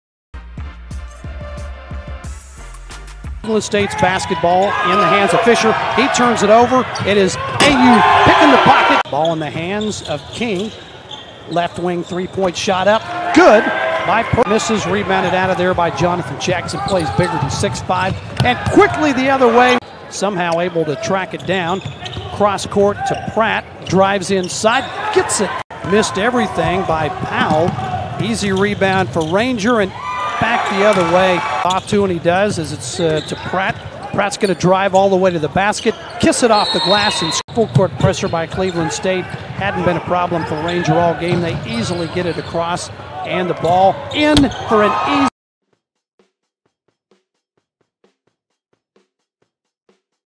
2019 NJCAA DI Men's Basketball Championship - Cleveland State vs. Ranger
No. 8 Ranger ended No. 24 Cleveland State's cinderalla run with a win victory to advance to the quarterfinal round of the 2019 NJCAA DI Men's Basketball Championship in Hutchinson, KS